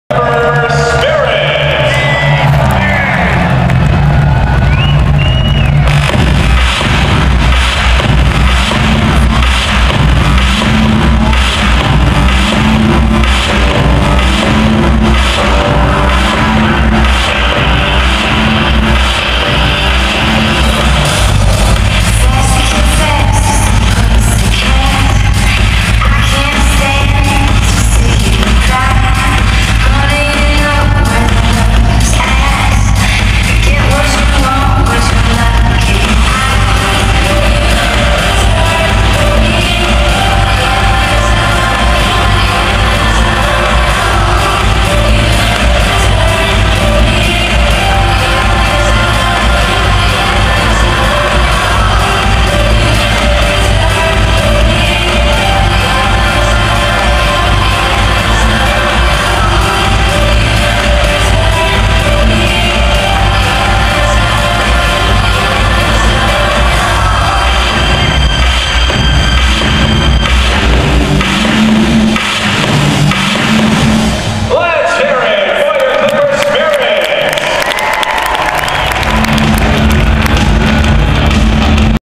L A Clippers Spirit Dancers -4th Quarter Dance
This Dance was soooooo good the Clippers came back from 12 down to win the game!! Amazing job girls!!